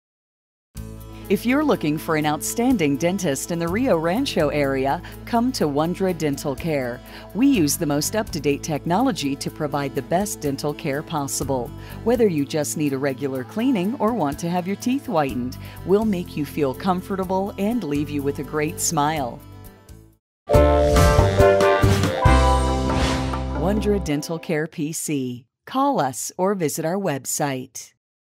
Wondra Dental Care PC
If youre looking for an outstanding dentist in the Rio
Rancho area, come to Wondra Dental care.  We use the most up
to date technology to provide the best dental care possible.
 Whether you just need a regular cleaning or want to have
your teeth whitened, well make you feel comfortable and
leave you with a great smile